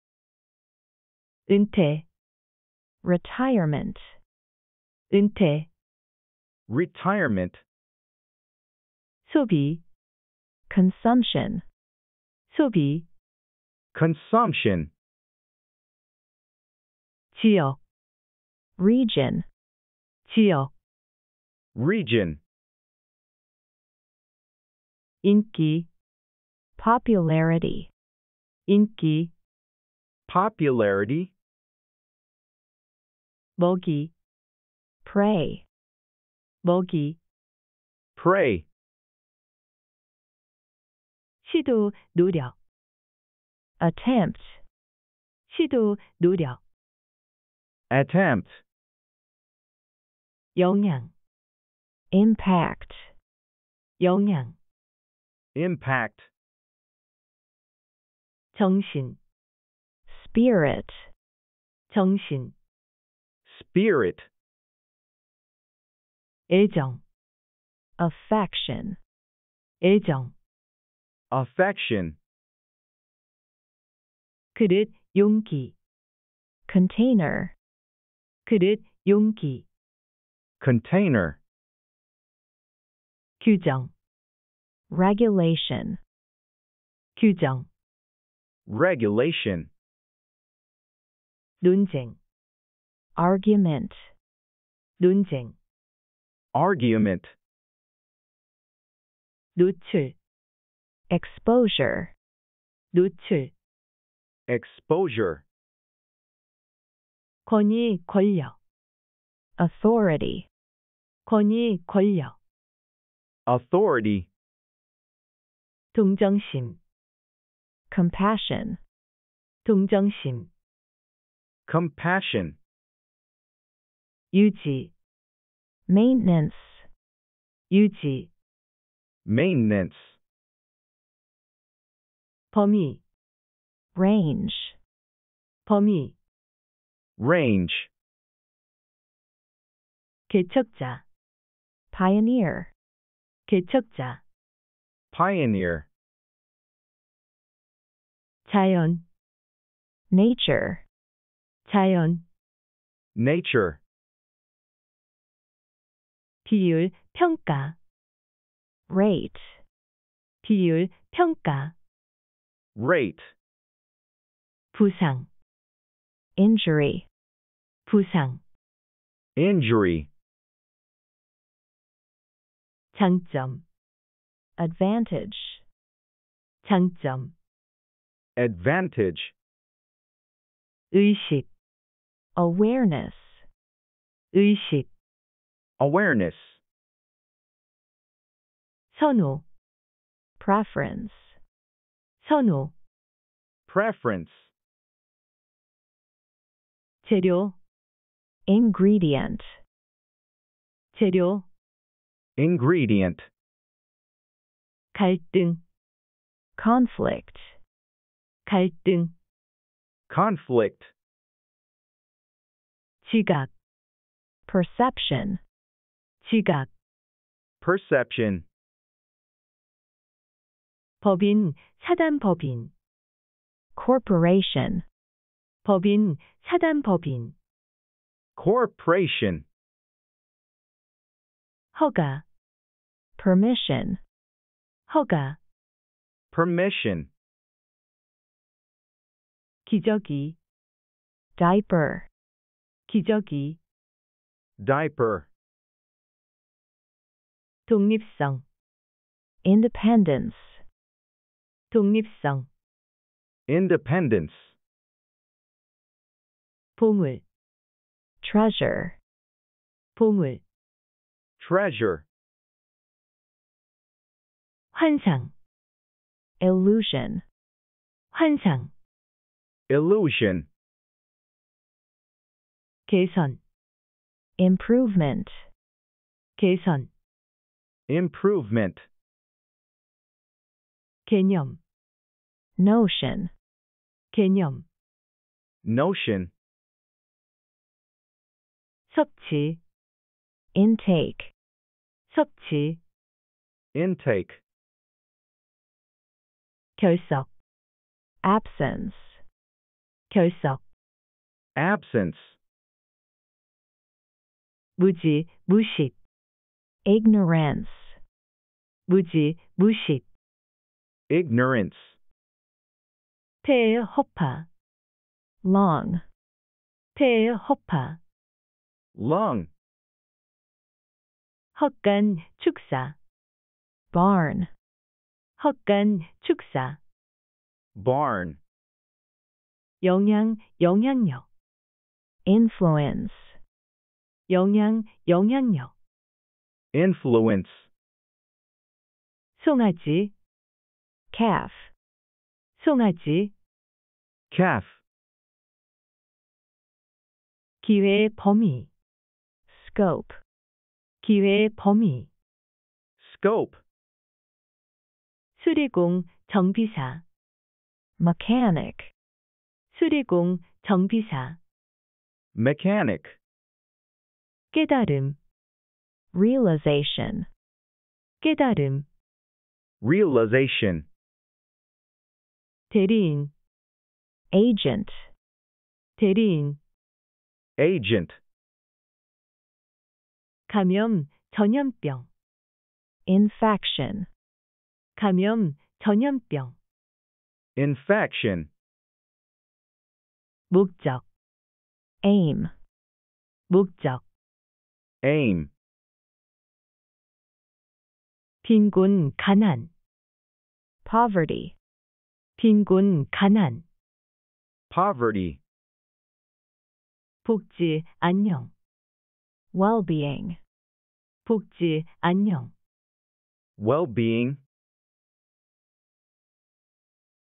#44 쉽게 외우는 영어단어 명사 매일 영어
은퇴-retirement
소비-consumption
지역-region
인기-popularity
먹이-prey
시도, 노력-attempt
영향-impact
정신-spirit
애정-affection
등 영어 단어 50개 입니다.